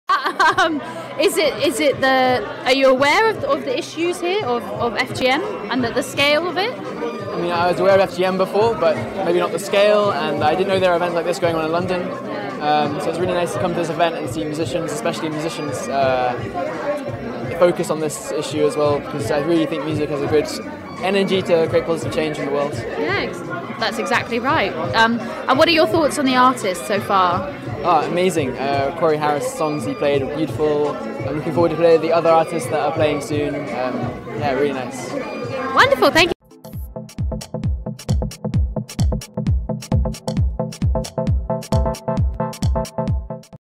Henry Ashworth - The Power of Music: International Women's Day by WinkBall
London based African women’s charity FORWARD who has campaigned on female genital mutilation (FGM) for over 30 years is hosting US Blues artist Corey Harris for a UK tour this March to mark International Women’s Day, as part of their Arts Council funded project Musicians Unite to End FGM.

FORWARD’s Musicians Unite to End FGM project engages with well-known artists to raise awareness of FGM among a wider audience and to audiences of artists whose countries of origin are affected by the issue.

Corey Harris has earned substantial critical acclaim as one of the few contemporary bluesmen able to channel raw, direct emotion of acoustic Delta blues. He appeared in the iconic Martin Scorsese 2003 film 'Feel Like Going Home' from The Blues Film Collection and has collaborated with the likes of BB King, Taj Mahal, Ali Farka Toure, Buddy Guy amongst others.

The UK tour of the Corey Harris Band includes an event on International Women’s Day (8th March) at Hackney Attic entitled The Power of Music: Artists using conversation and music for social change. This event sees Corey Harris along with NKA1 and Tunde from Congolese hip hop group Lopango ya Banka and UK based rapper Sharna Cane, being interviewed by journalist Ade Daramy on the power of music to create positive social change and raise awareness of issues such as FGM, followed by live performances from Nka1 and Sharna Cane.

Co-hosted with Black HistoryStudies: The Power of Music, Artists using conversation and music for social change feat. Journalist Ade Daramy, Nka1 and Tunde (Lopango Ya Banka, Lopango Ya Banka), Corey Harris & Sharna Cane 14,  Hackney Picturehouse (HackneyAttic)